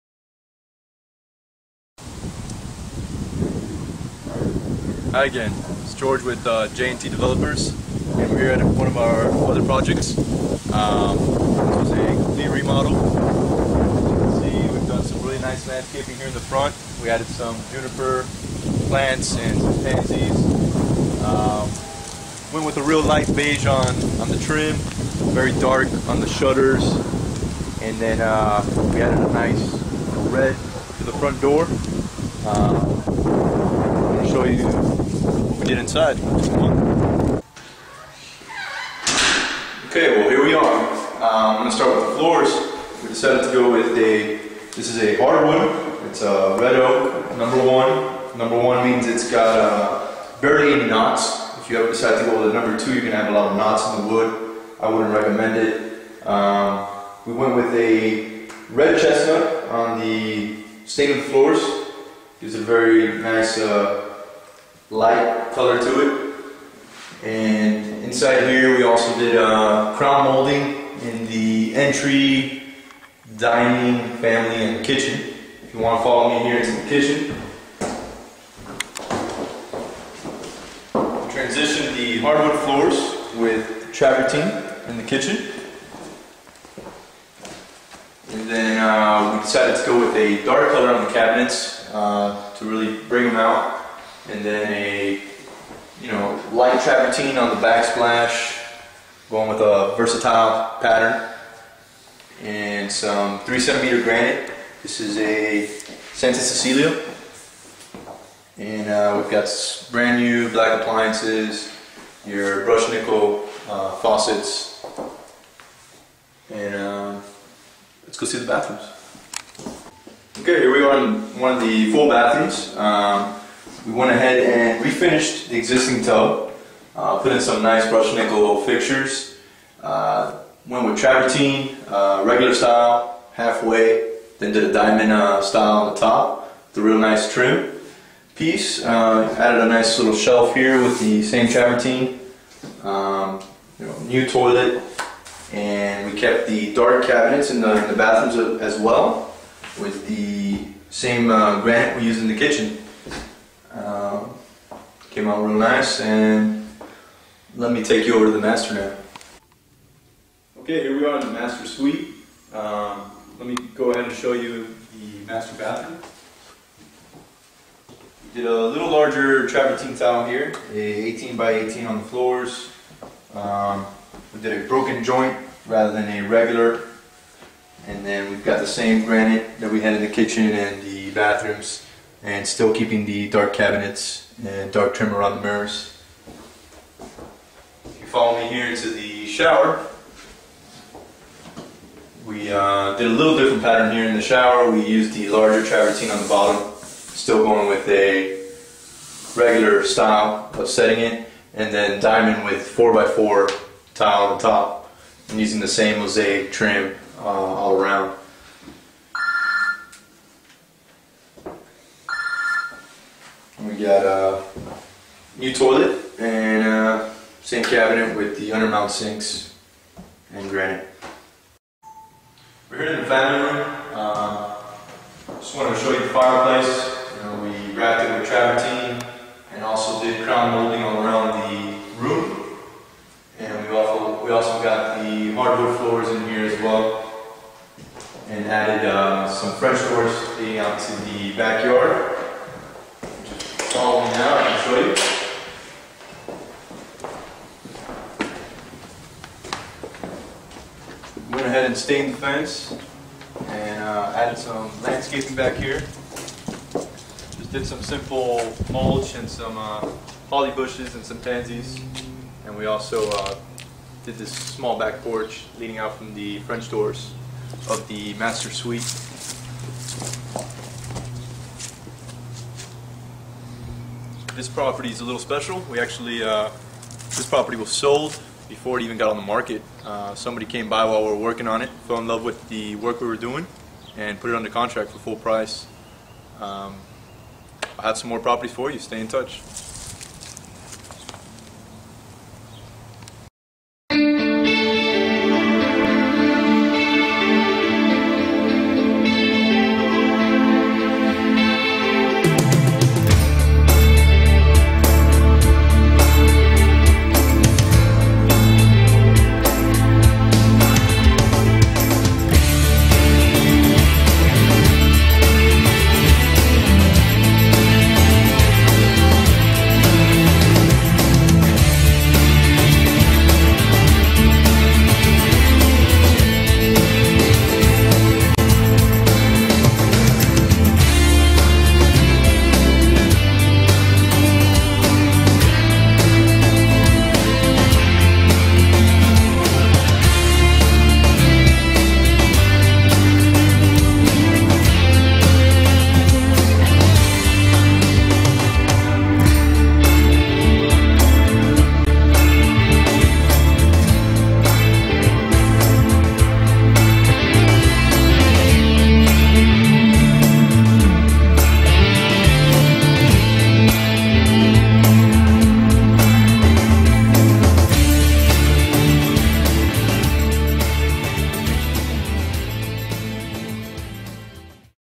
Roofing Contractors Residential Contractors Dallas TX
8360 LBJ Freeway
Suite #270

This is an example of what a full remodel looks like.  We have included some before screenshots to show you the conditions this nice East Dallas Home was in.  It too us but one day to list it and have it sold.